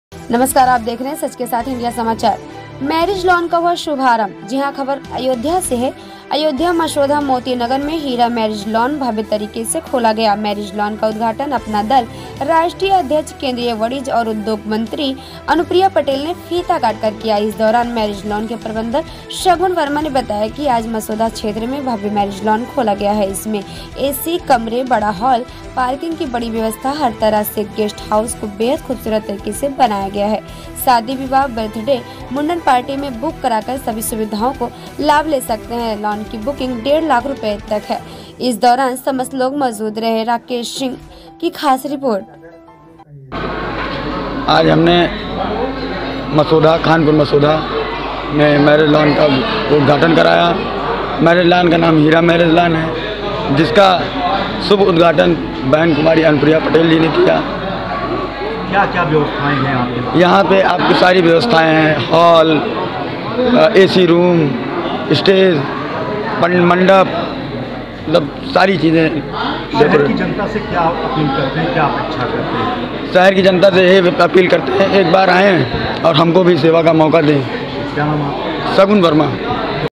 मसौधा मोती नगर में हीरा मैरिज लान भव्य तरीके से खोला गया। मैरिज लान का उद्घाटन अपना दल
मैरिज लान का हुआ शुभारंभ

अयोध्या
अयोध्या। मसौधा मोती नगर में हीरा  मैरिज लान भव्य तरीके से खोला गया। मैरिज लान का उद्घाटन अपना दल राष्ट्रीय अध्यक्ष व केंद्रीय वाणिज्य औऱ  उद्योग राज्यमंत्री अनुप्रिया पटेल ने फीता काटकर किया। इस दौरान मैरिज लान के प्रबंधक शगुन वर्मा ने बताया कि आज मसौधा क्षेत्र में भव्य मैरिज लान खोला गया है इसमें एसी कमरे, बड़ा हाल , पार्किंग की बड़ी व्यवस्था हर तरह से गेस्ट हाउस को बेहद खूबसूरत तरीके से बनाया गया है। शादी विवाह, बर्थडे ,मुंडन पार्टी में  बुक करा कर सभी सुविधाओं का लाभ लें सकते है। लान की बुकिंग 151000 रुपए है। इस दौरान समस्त लोग मौजूद रहे।

राकेश सिंह